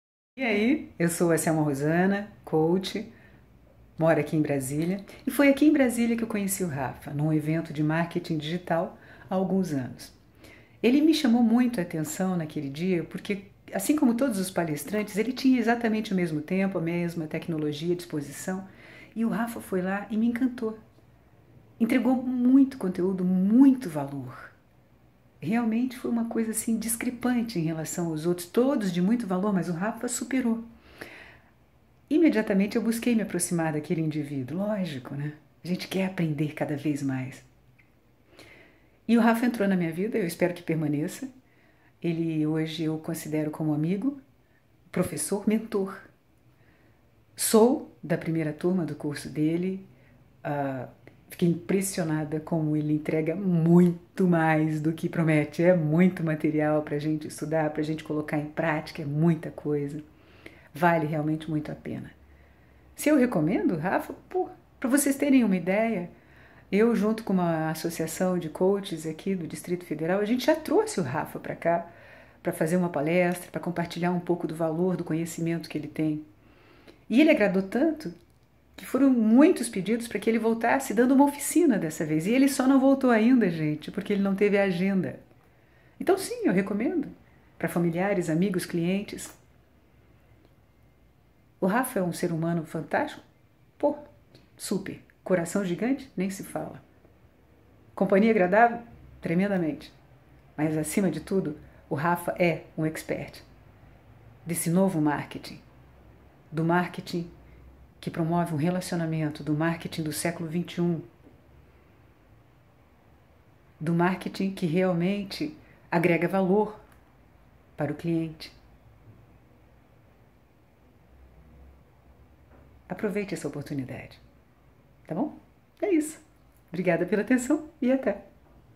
Depoimento Selma Rosana
Depoimento da Coach Selma Rosana sobre o Curso Online de Marketing de Conteúdo